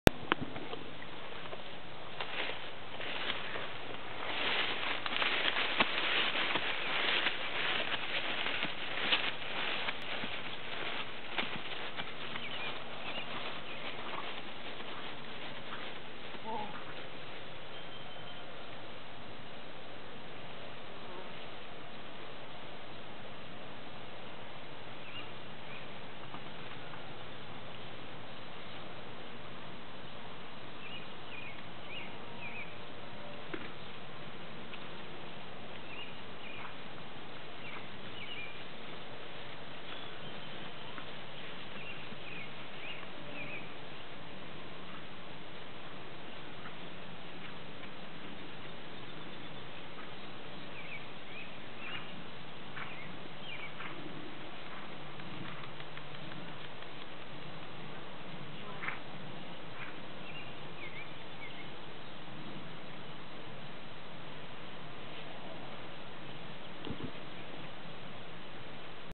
Haven't ridden in a month. . .
It's been a month since I've been able to ride. . . opted to go WITHOUT the saddle. . . short clip cause the battery ran out on the camera